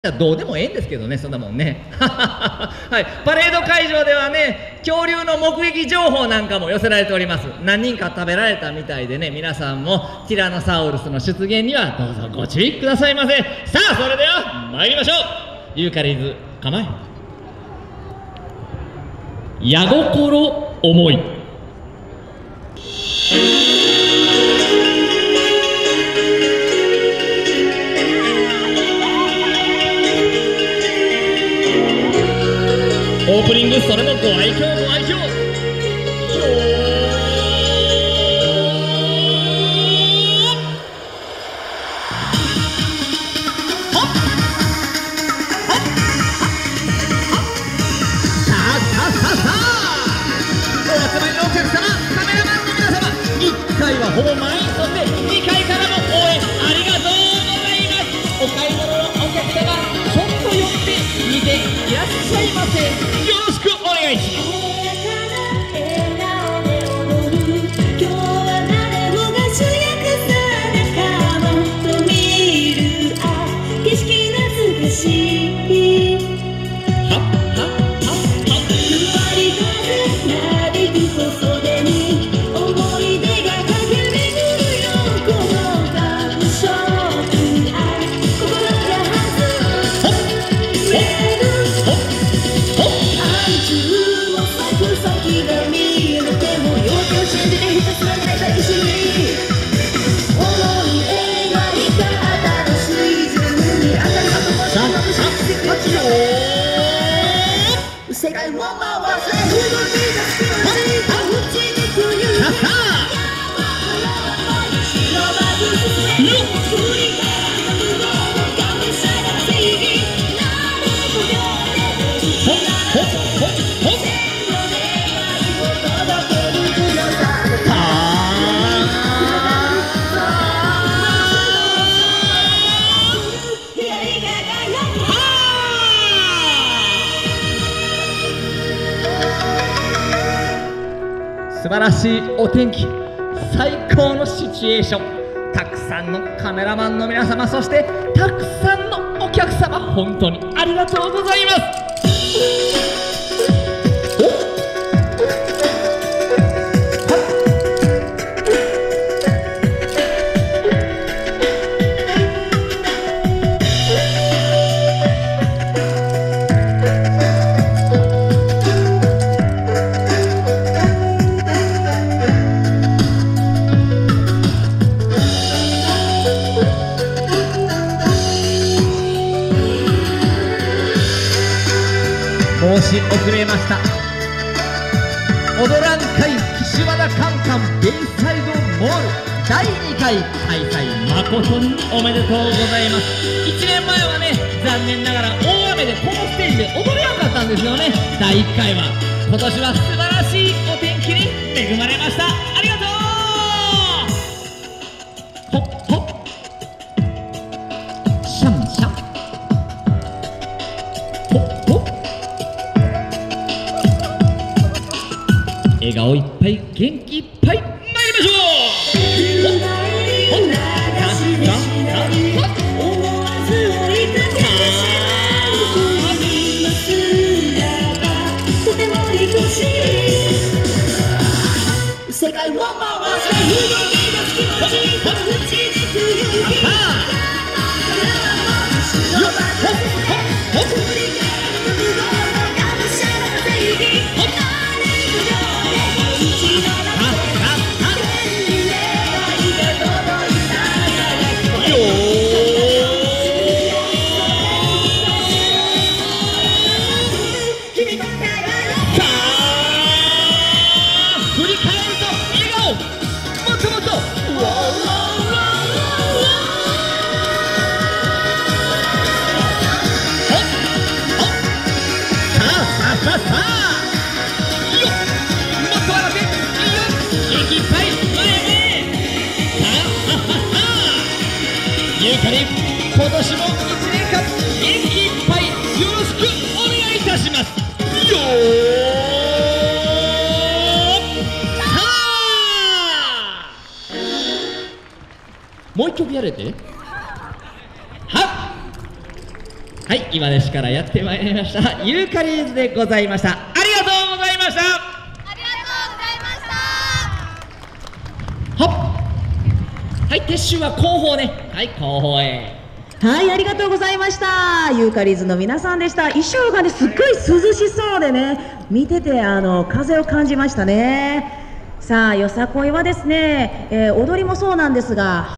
[4K] ゆうかり～ず ① 踊らんかい!!岸和田よさこいBAYSIDE!! 2024 (中央)
2024年4月14日(日)
踊らんかい!!岸和田よさこいBAYSIDE!!
岸和田カンカンベイサイドモール